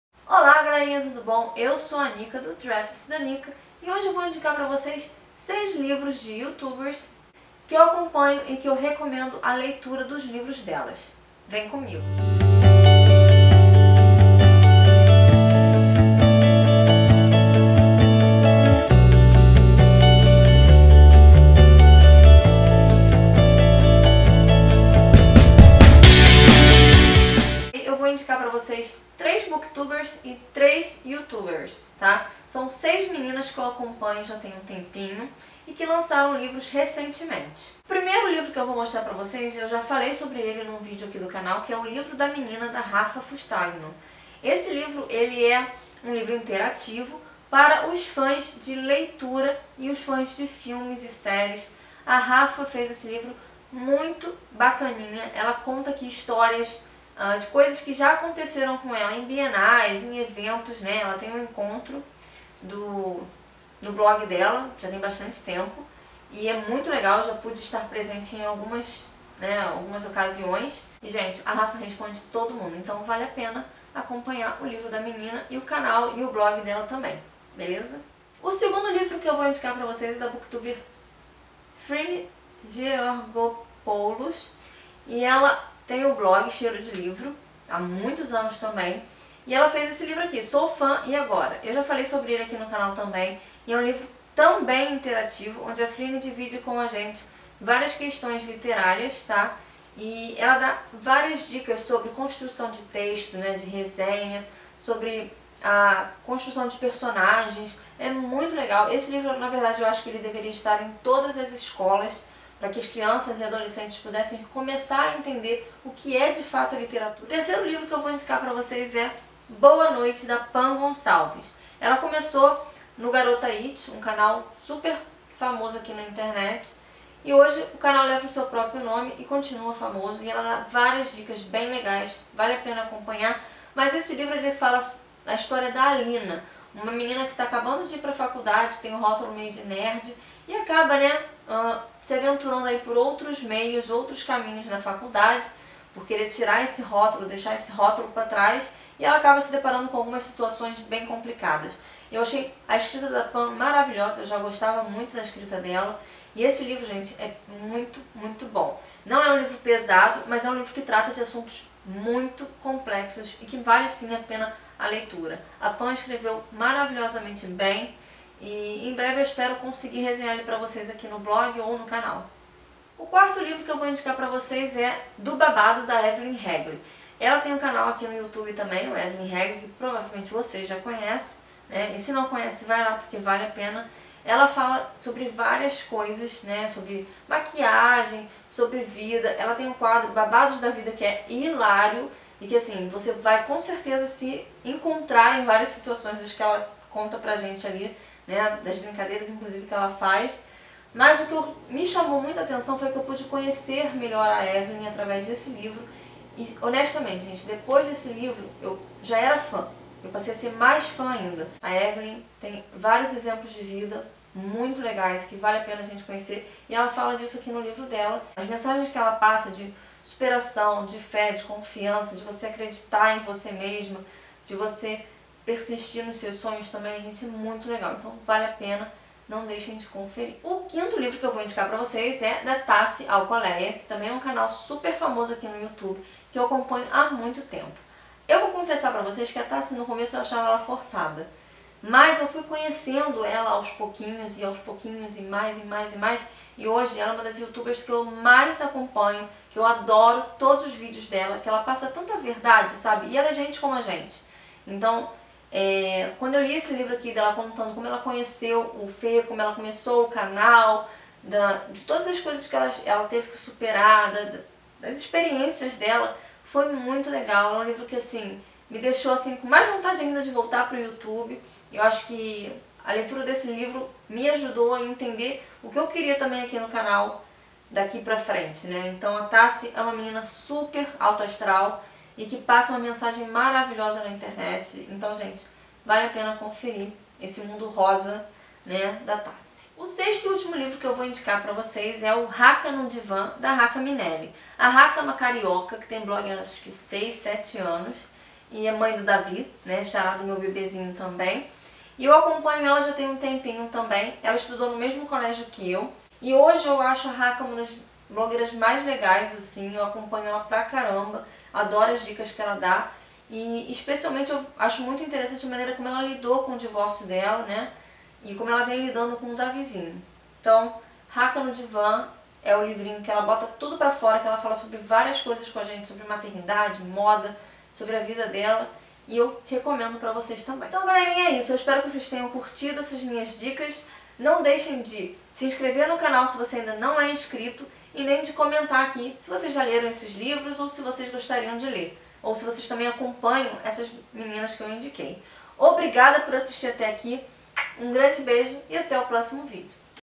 GIRL POWER | Youtubers que eu indico
Hoje vou indicar pra vocês seis YouTubers, entre elas três booktubers, que eu recomendo a leitura dos seus livros!

Quer nos enviar produtinhos para resenha? Basta mandar para:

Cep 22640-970

Música da biblioteca do IMovie.